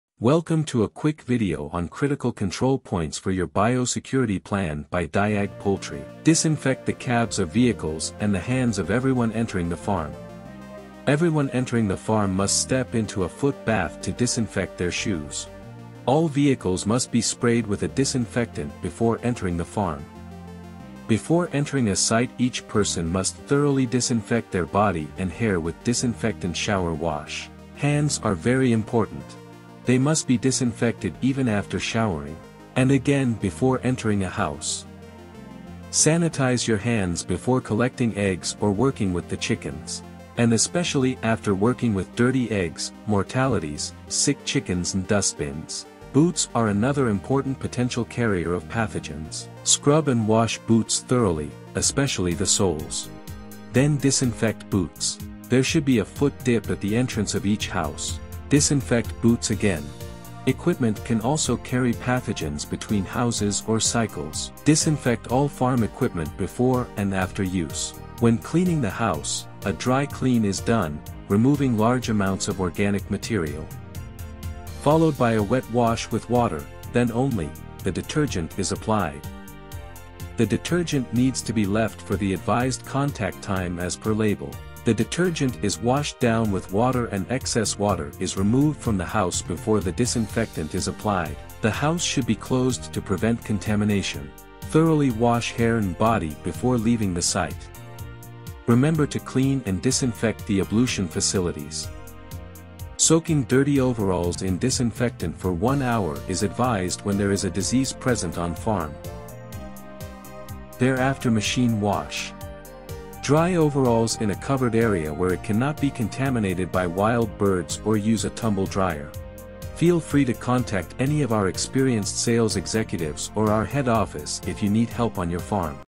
Critical control points, with products you can use, at these points - Bio-security by DIAG Poultry +
This video is to help identify critical control points for your Bio - security program on your chicken farm.